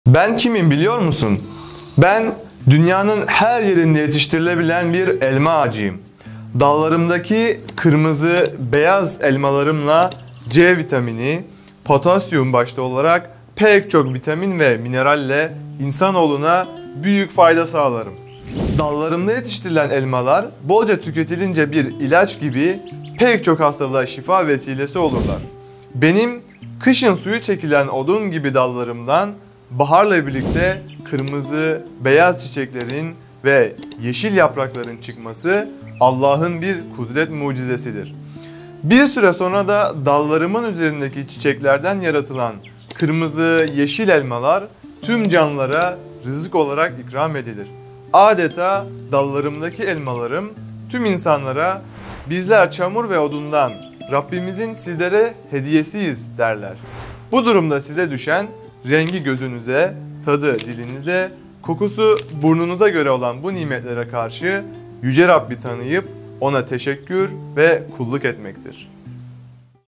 Ben kimim biliyor musun?
Ben dünyanın her yerinde yetiştirilebilen bir elma ağacıyım. Dallarımdaki kırmızı - beyaz elmalarımla C vitamini, potasyum başta olarak pek çok vitamin ve mineralle insanoğluna büyük fayda sağlarım. Dallarımda yetiştirilen elmalar, bolca tüketilince bir ilaç gibi pek çok hastalığa şifa vesilesi olurlar.Benim kışın suyu çekilen odun gibi dallarımdan baharla birlikte kırmızı, beyaz çiçeklerin ve yeşil yaprakların çıkması Allah'ın bir kudret mucizesidir. Bir süre sonra da dallarımın üzerindeki çiçeklerden yaratılan kırmızı, yeşil elmalar tüm canlılara rızık olarak ikram edilir.Âdeta dallarımdaki elmalarım tüm insanlara, ‘bizler çamur ve odundan, Rabbimizin sizlere hediyesiyiz’ derler. Bu durumda size düşen; rengi gözünüze, tadı dilinize, kokusu burnunuza göre olan bu nimetlere karşı Yüce Rabbi tanıyıp O’na teşekkür ve kulluk etmektir.